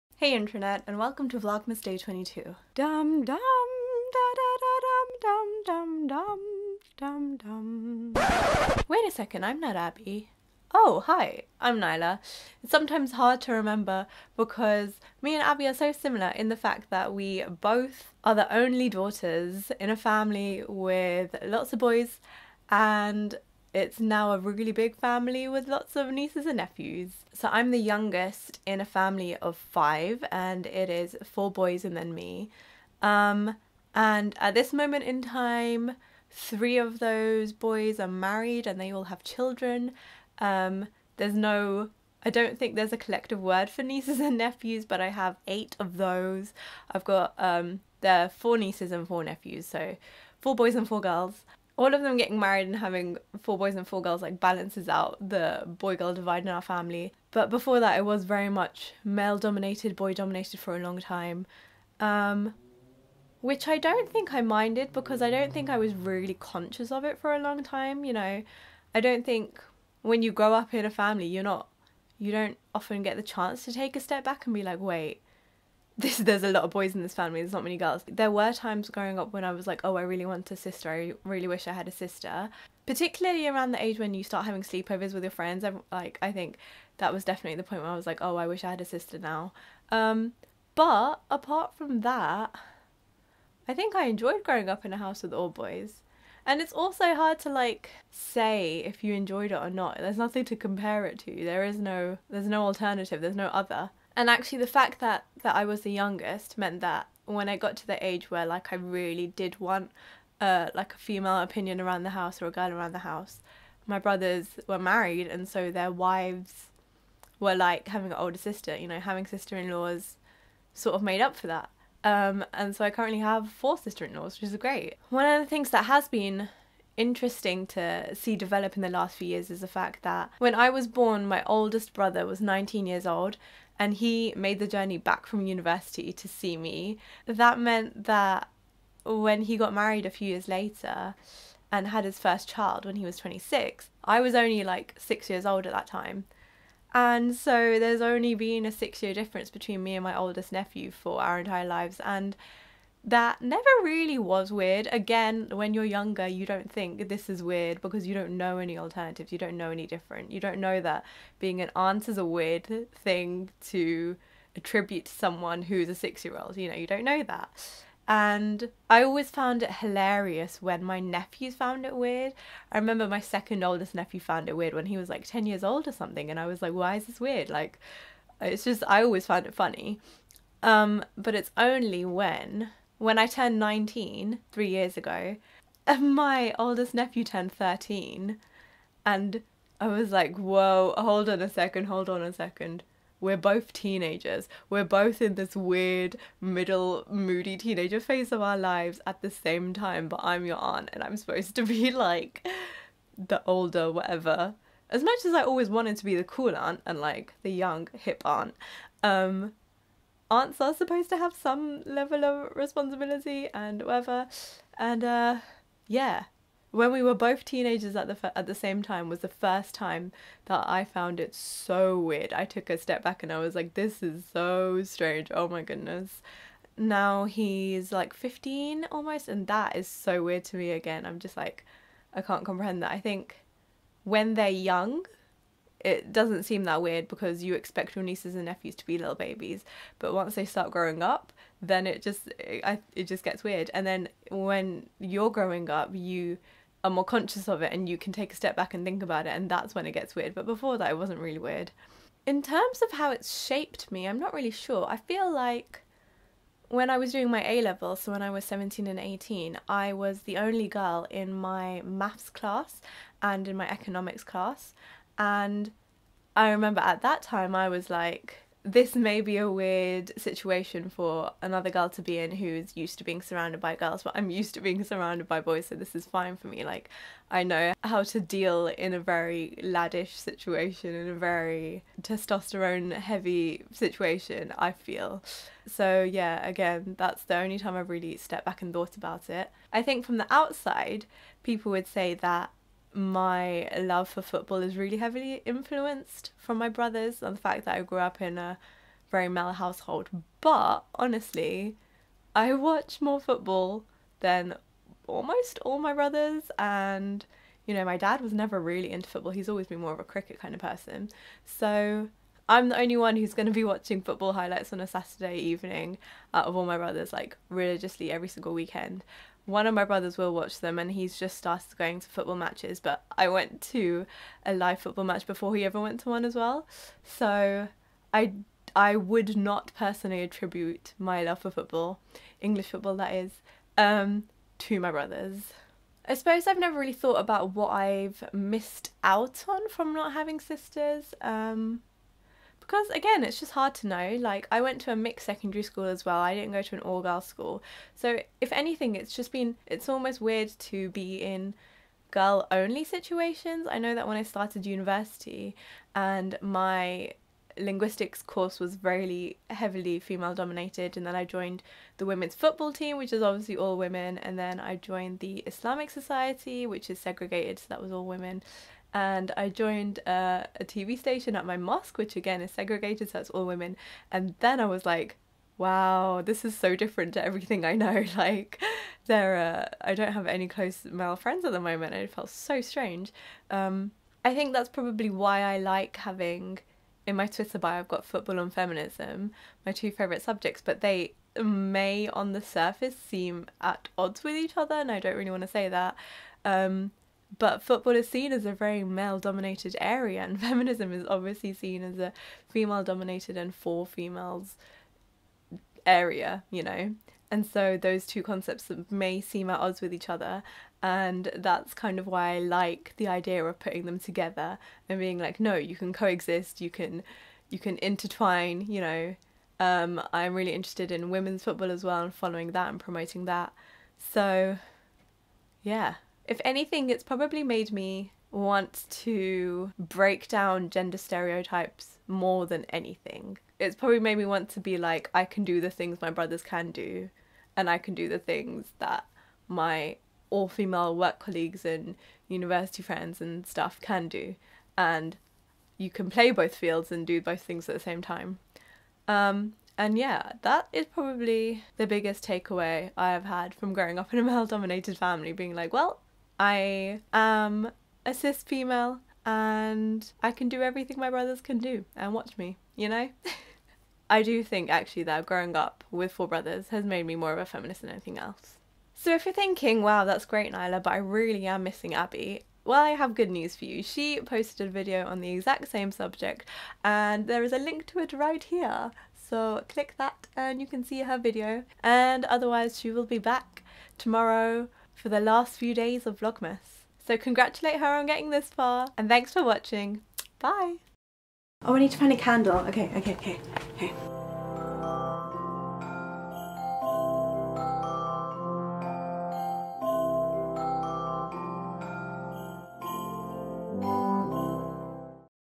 Family | Vlogmas Day 22
For today's vlogmas, we have a special guest: Nylah We discovered that we both are the only girl in a family of boys, as well as aunts, so we swapped channels to talk about our experiences. If you would like to hear my thoughts, you can watch my video on her channel